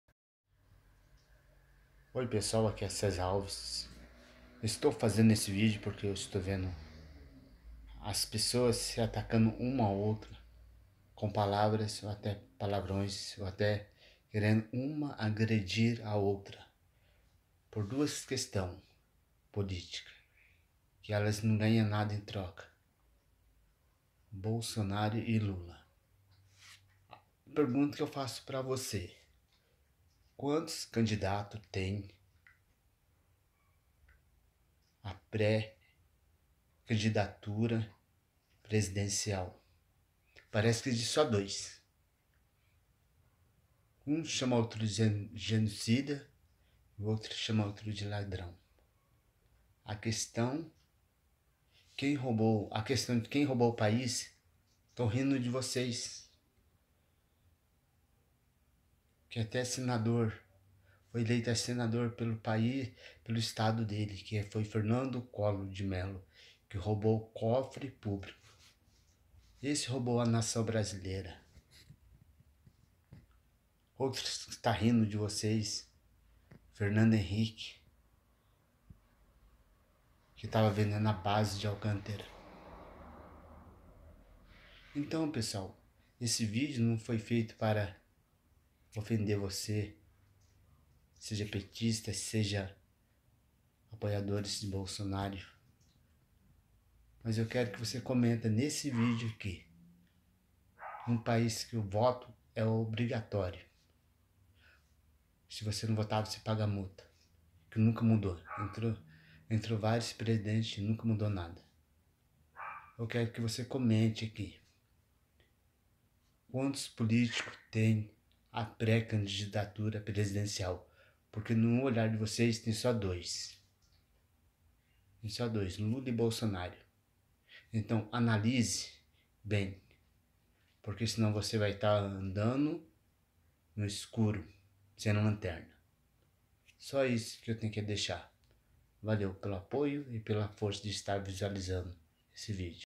Bolsonaro vs LuLa . e outros políticos
APENAS MENCIONADO BOLSONARO E LULA.
PARA QUE TEM APENAS DOIS PRÉ CANDIDATO A PRESIDÊNCIA.
NAÇÃO BRASILEIRA ANDA DE OLHOS FECHADO.
EXISTE VARIOS PRÉ CANDIDATOS.
QUEM ROUBOU A NAÇÃO BRASILEIRA E OS COFRE PUBLICO. CHAMA-SE.
FERNANDO COLLOR DE MELO.
ESSE RIR DA PESSOAS QUE ESTÃO
MENCIONANDO APENAS.
BOLSONARO E LULA.
INFELIZMENTE  AS PESSOAS ESTÃO ANDANDO NO ESCURO DE OLHOS FECHADOS E SEM LATERNA.
INFELIZMENTE COM ESSAS MENTALIDADES.
O BRASIL TEM SÓ A PERDER.
AS DEVERIA SE UNIR.
MAIS SÃO OPOSTAS COM A MESMA CEGUEIRAS.
FAÇA O SEU COMENTÁRIO ABAIXO.